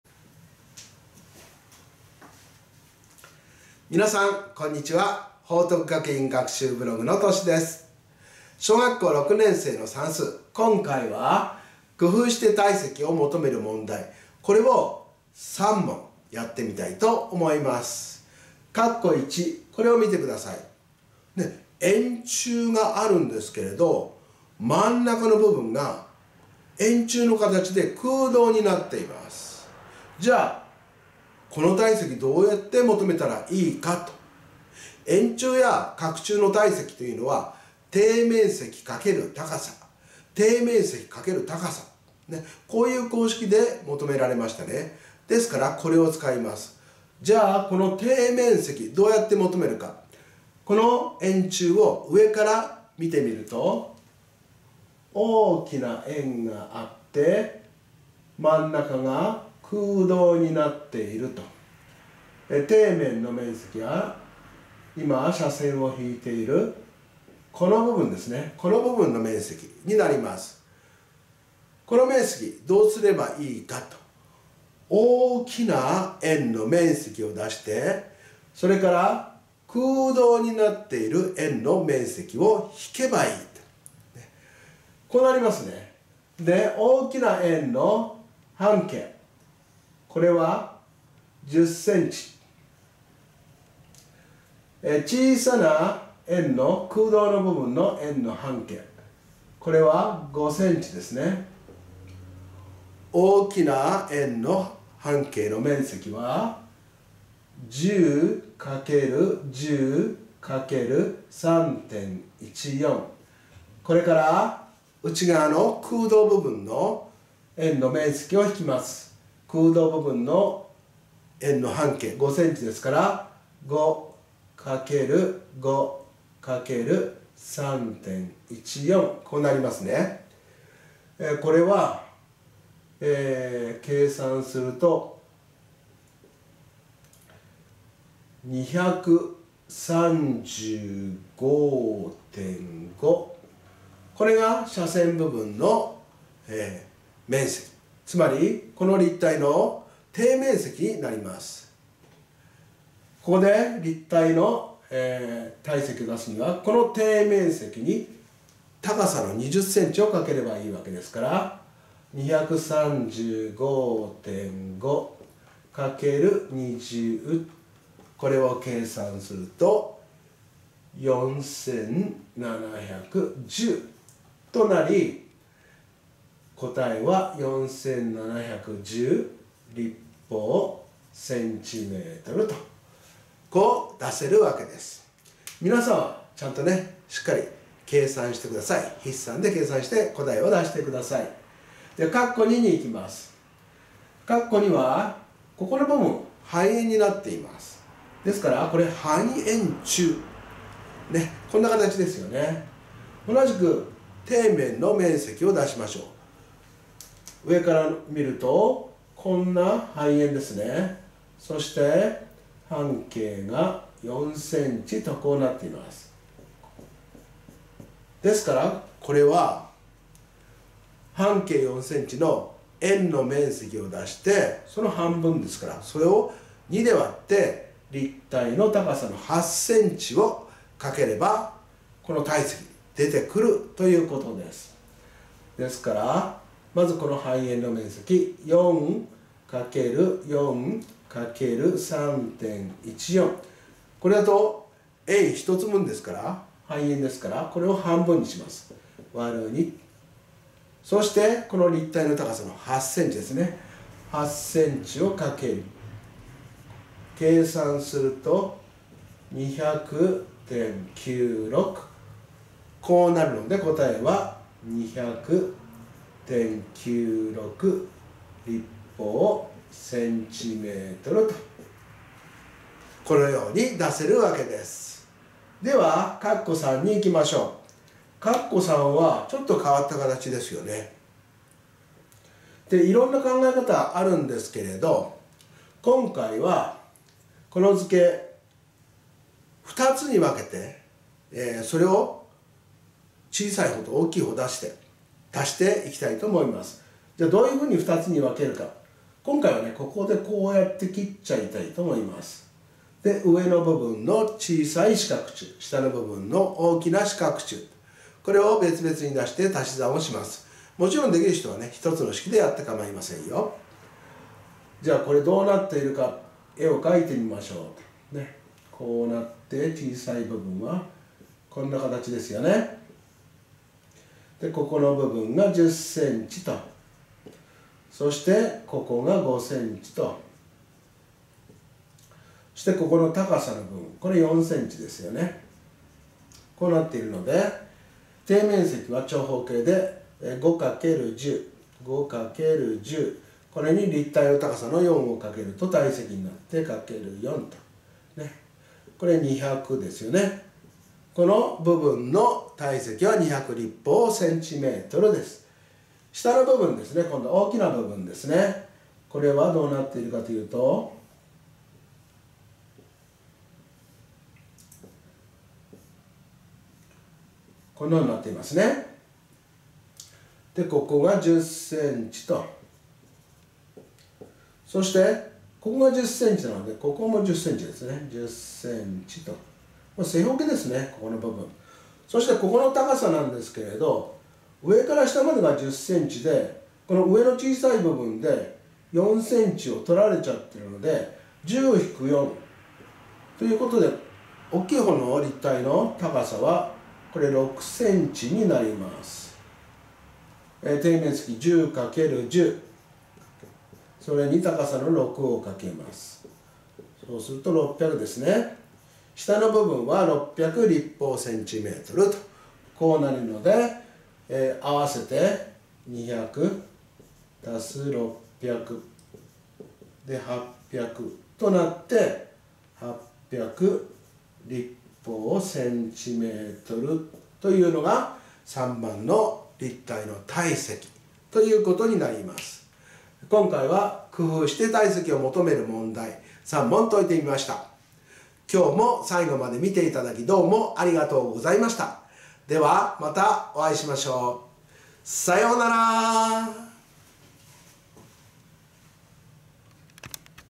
くふうして体積を求める問題【小6算数】
円柱の中に空洞の円柱のある場合、半円柱の体積、四角柱が重なっている場合の体積を求める問題です。